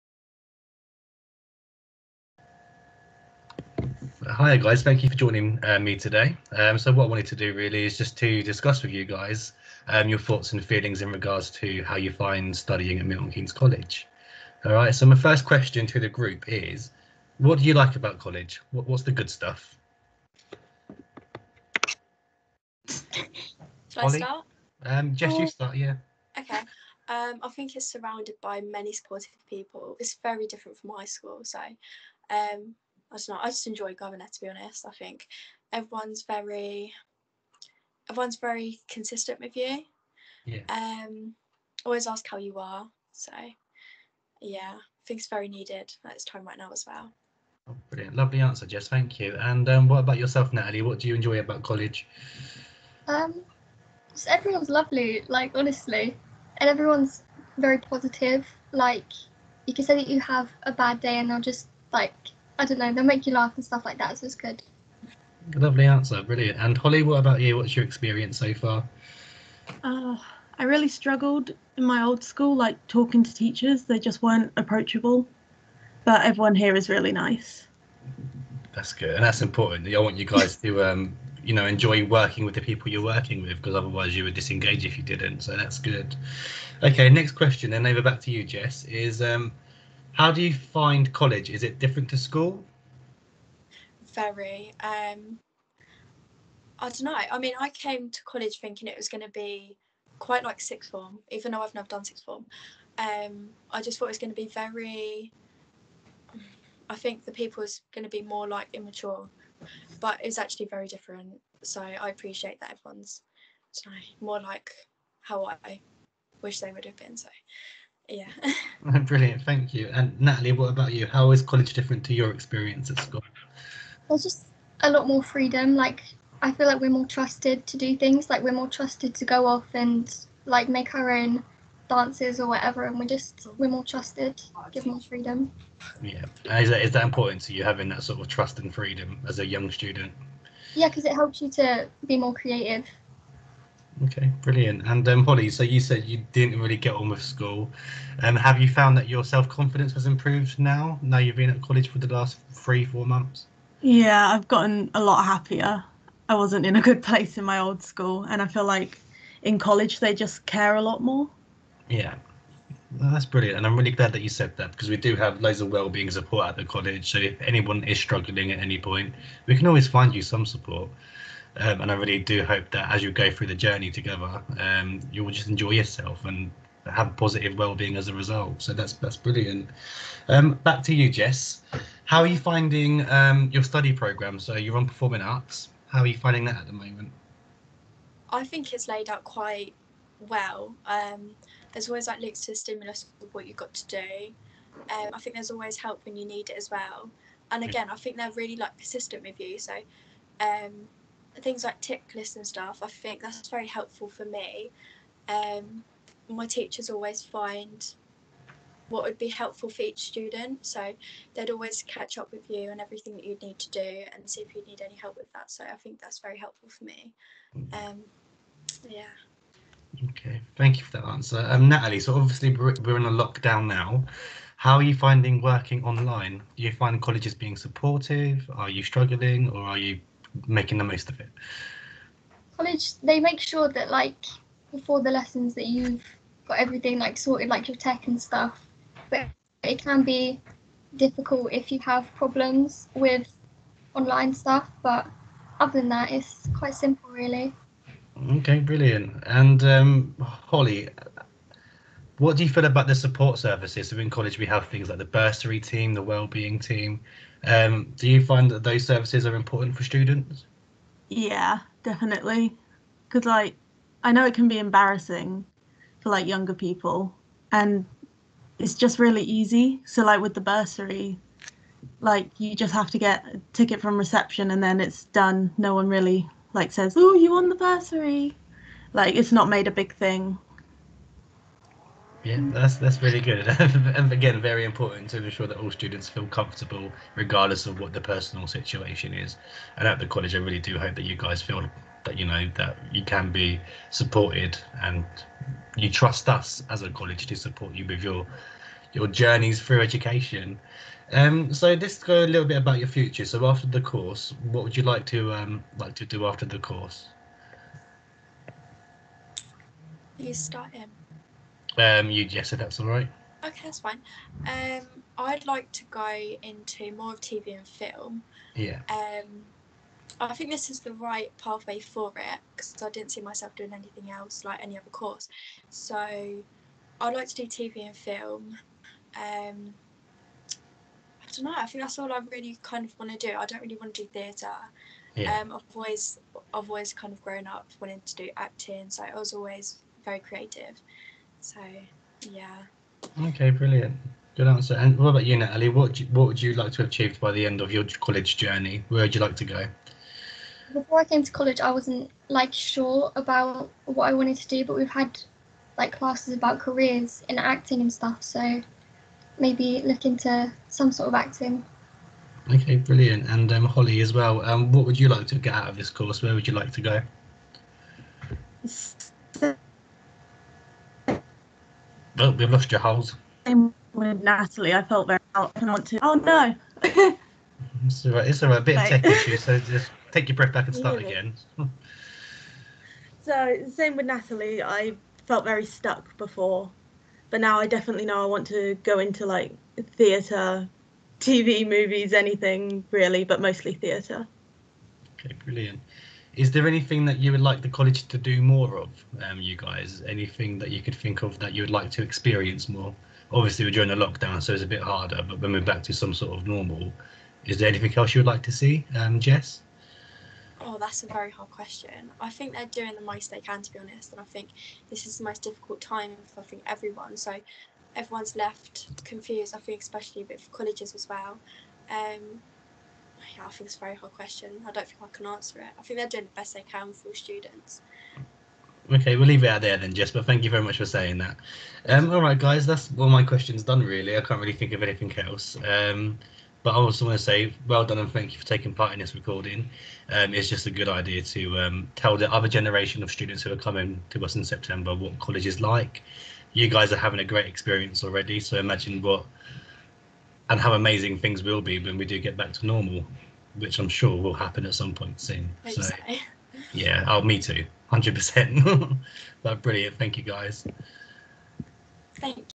Student View
Listen to Colin, a tutor on our Performing Arts course, chatting with his students. Jess, Natalie and Holly will give their experiences of coming to College, support offered to learners and studying during lockdown.